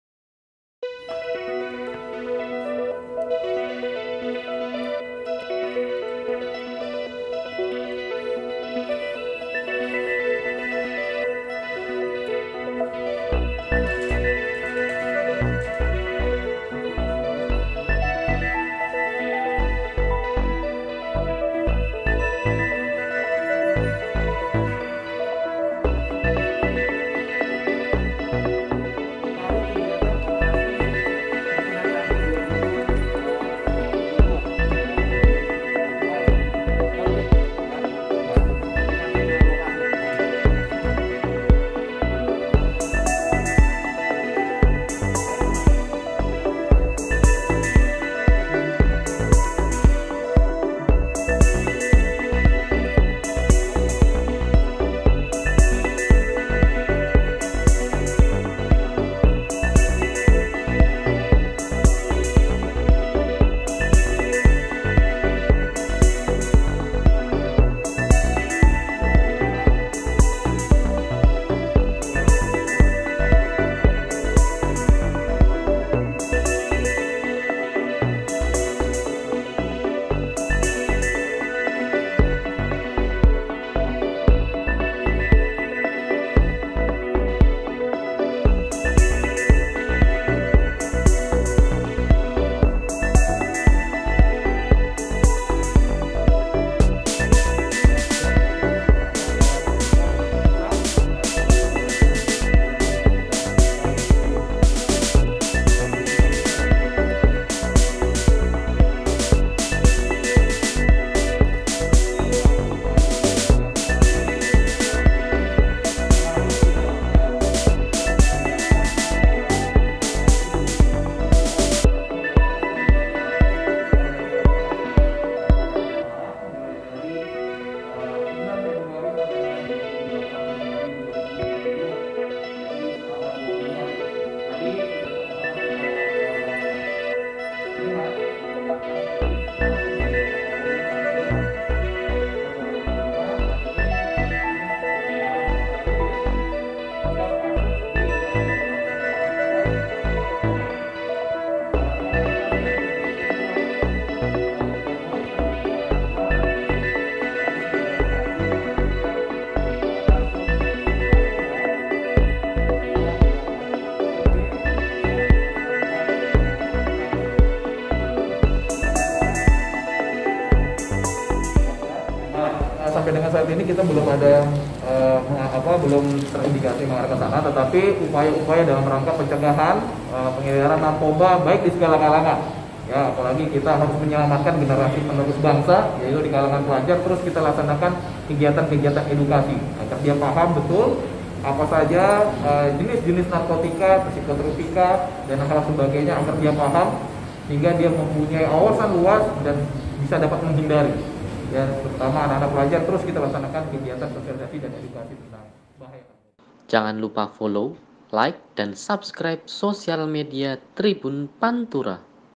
Polisi Gagalkan Nelayan Pesta Sabu di Rumah Kos Pemalang
Polres Pemalang berhasil mengamankan 8 orang tersangka dari 4 kasus tindak pidana penyalahgunaan narkotika, yang terungkap selama berlangsungnya operasi bersinar candi tahun 2022.

Dari, 4 orang tersangka di antaranya sebagai nelayan, mereka diamankan saat hendak menyalahgunakan narkotika jenis sabu di dalam kamar kos yang berlokasi di Kel Pelutan.

Setelah dilakukan penggeledahan, anggota Satresnarkoba menemukan 2 paket narkotika jenis sabu, lengkap dengan alat hisab.